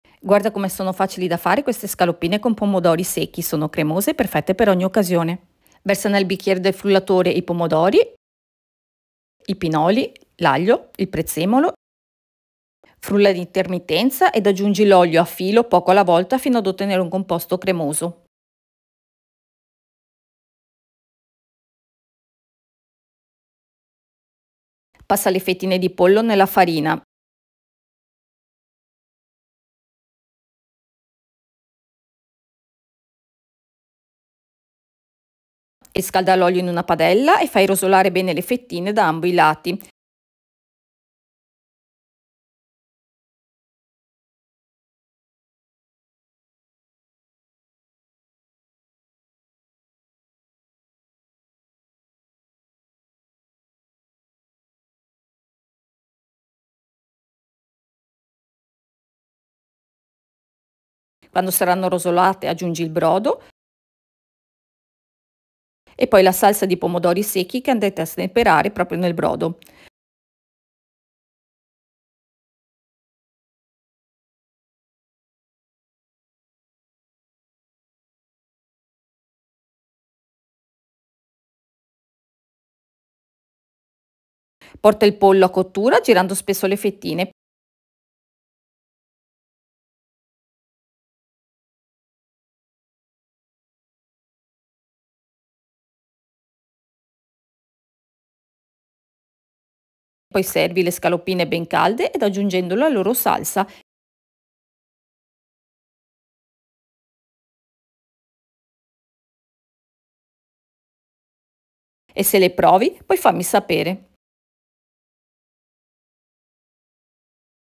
Scaloppine con pomodori secchi
Scaloppine con pomodori secchi un secondo piatto semplicissimo fare perfetto anche per le grandi occasioni. Le scaloppine di pollo con pomodori secchi sono cremose e le potete preparare anche con delle fettine di vitello se le preferite al pollo. Per preparare le fettine di carne con i pomodori secchi ho usato dei pomodori secchi sott’olio, se non avete quelli sott’olio, potete mettere i vostri pomodori secchi in una ciotola ed aggiungere dell’olio e li lascerete così per una mezz’ora così che il pomodoro possa assorbire l’olio ed ammorbidirsi oltre che prendere più sapore. Neanche voi amate le scaloppine vi consiglio di non perdere la mia raccolta di scaloppine per ogni occasione. Ora andiamo a vedere come fare le scaloppine con pomodori secchi e se le provate fatemi sapere. Barbara

ATTIVA LE NOTIFICHE CLICCANDO SULLA CAMPANELLA PER ESSERE AVVISATO OGNI VOLTA CHE PUBBLICO UNA NUOVA RICETTA

INGREDIENTI :
4 fette petto di pollo
100 g pomodori secchi sott’olio
10 g pinoli
q.b. prezzemolo
q.b. aglio
q.b. olio extravergine d’oliva
100 ml brodo vegetale